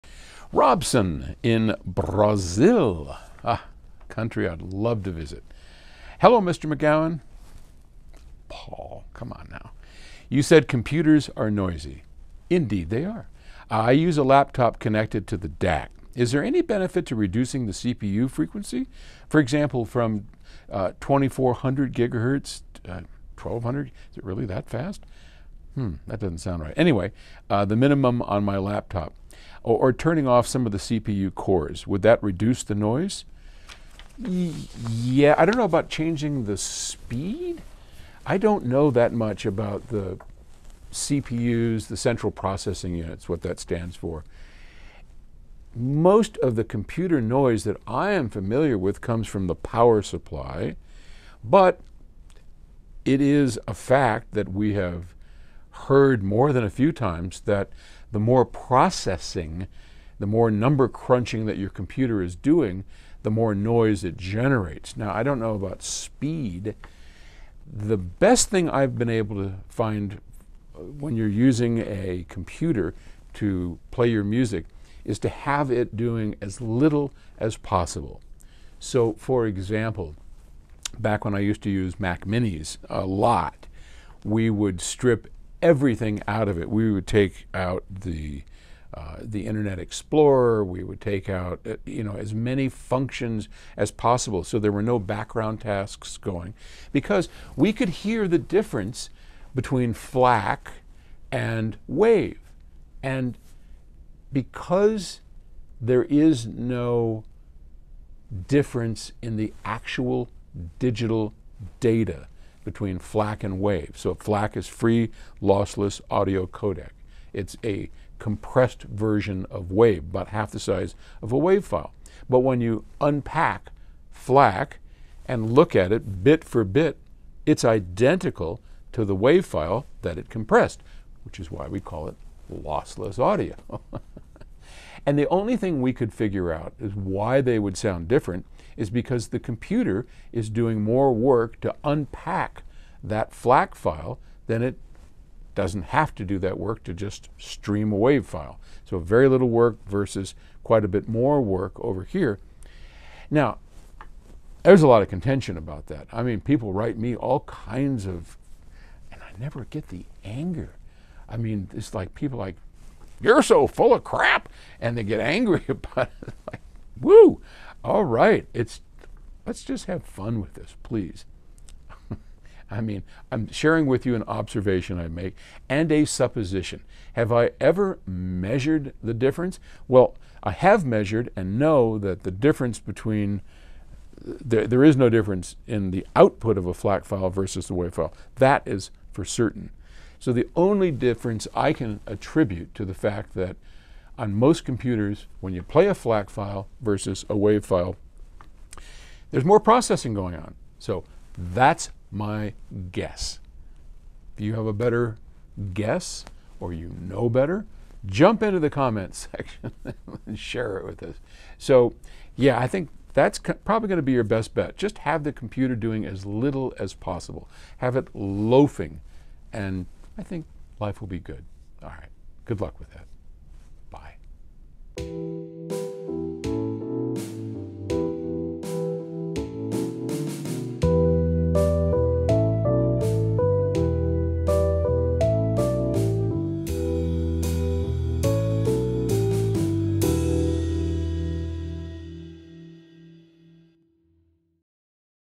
Changing CPU speed for better audio
Is it possible to lower digital noise by changing the CPU speed?